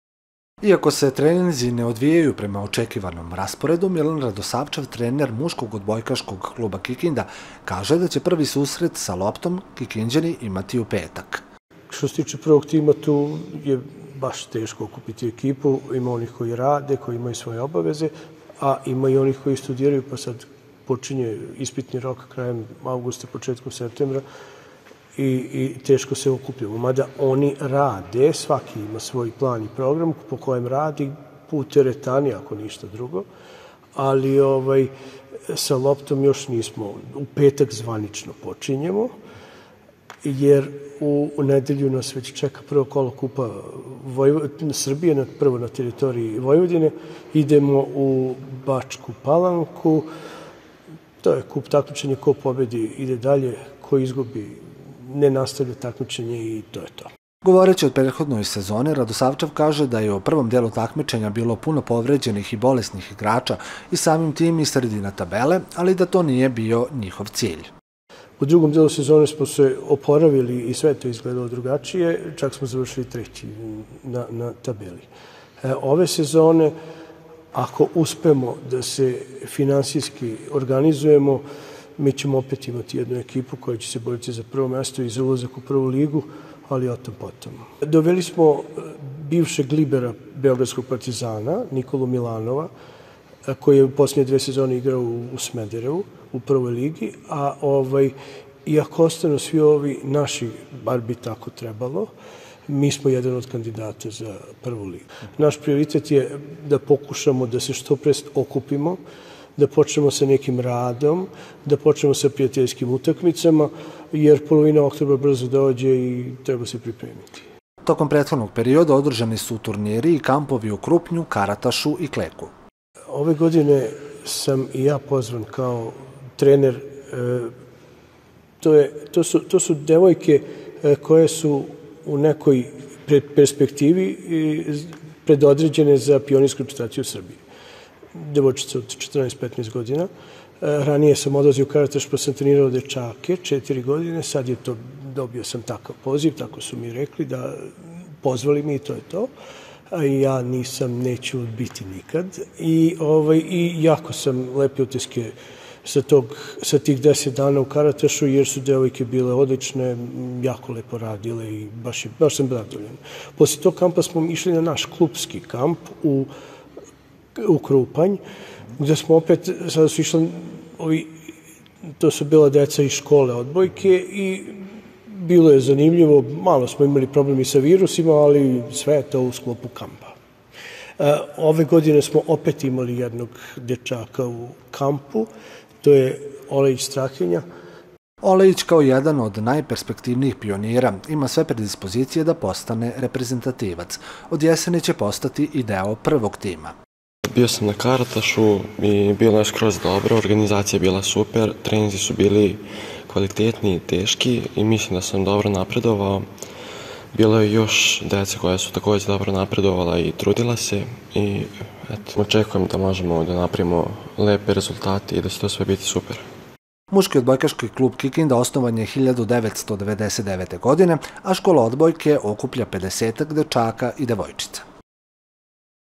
Odbojkaši MOK ,,Kikinde" polako se spremaju za početak nove sezone
Kako teku pripreme odbojkaša  pred početak nove sezone Druge lige sever koja počinje u drugoj polovini oktobra  saznajemo od trenera Milana Radosavčeva.

17 godina uz Vas